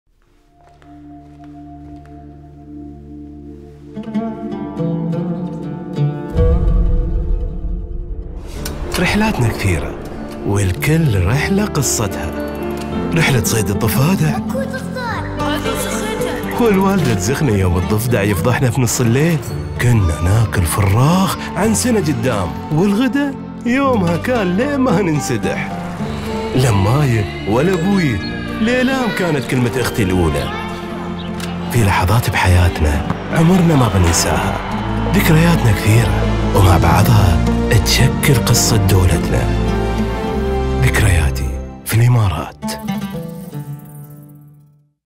اليوم الوطني 48
من رحلاتنا الطويلة للبر أو لمّتنا الحلوة ويا الأهل والأصدقاء، ذكرياتنا في البلاد شكلّت قصة وطننا. شاركونا ذكرياتكم بهاشتاق ذكرياتي_في_الإمارات وخلونا نعيش وياكم لحظات جميلة عشتوها في الدولة. نهنئكم باليوم الوطني 48 لدولتنا الحبيبة .